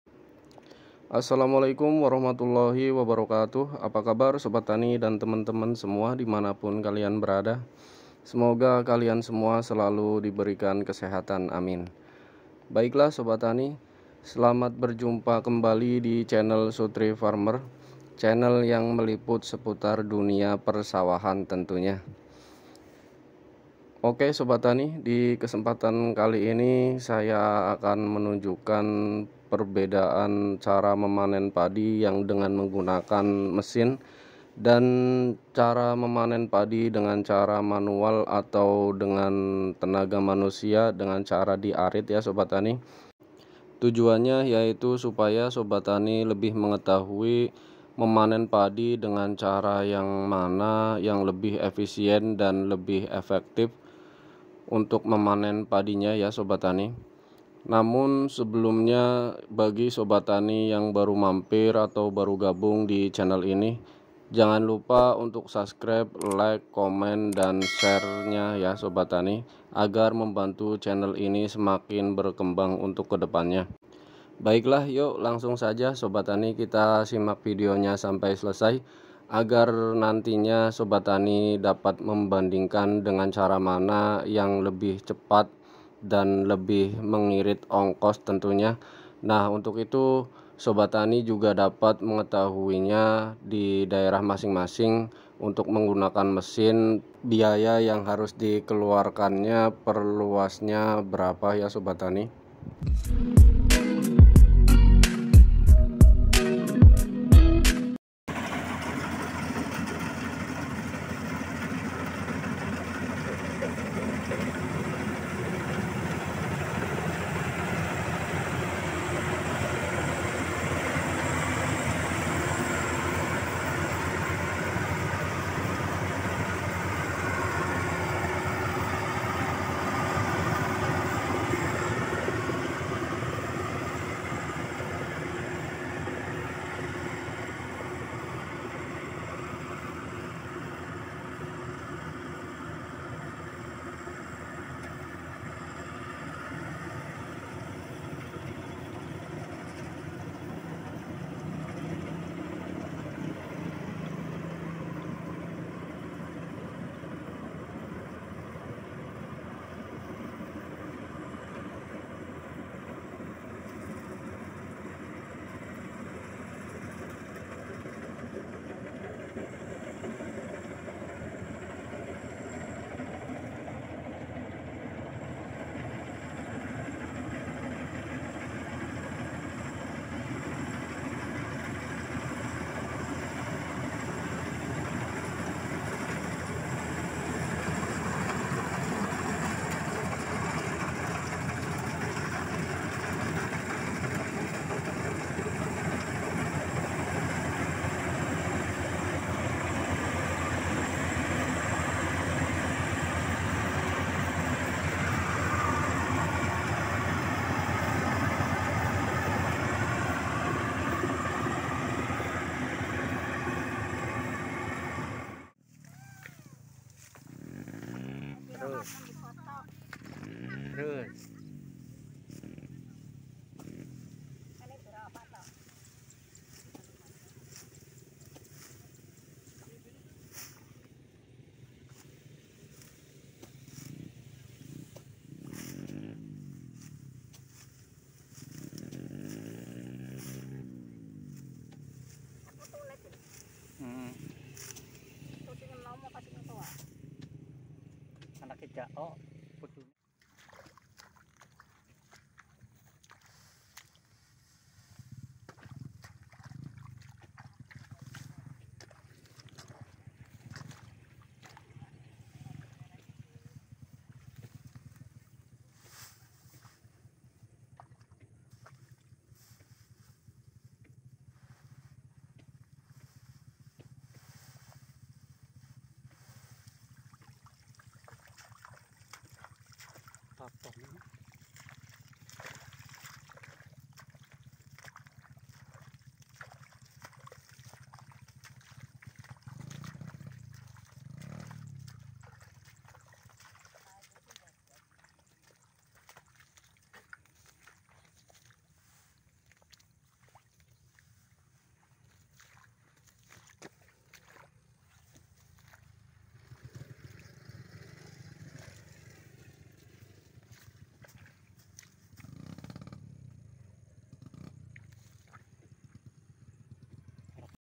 Lebih efektif mana !! Cara panen padi modern & cara panen padi tradisional
Di Vidio kali ini saya akan menunjukan perbedaan cara panen padi mana yang lebih efektif antara cara panen padi modern & cara panen padi tradisional. cara mana yang tentunya lebih mengirit biaya atau cost dalam proses panen padi di sawah. Menurut saya ini penting di ketahui oleh para petani padi agar mendapatkan keuntungan di tengah melonjaknya harga pupuk dan obat-obatan untuk perawatan tanaman padi. Di jaman serba modern seperti sekarang panen padi pun menggunakan teknologi atau panen padi modern yaitu dengan mesin combine Harvest. Namun masih banyak juga yang menggunakan cara manual atau panen padi tradisional yaitu dengan tenaga buruh tani..nah untuk mengetahui panen padi dengan cara mana yang lebih mengirit biaya,silahkan simak Vidio ini sampai selesai yaa? dan jangan lupa untu SUBSCRIK,LIKE,KOMENTAR Vidio ini yaa..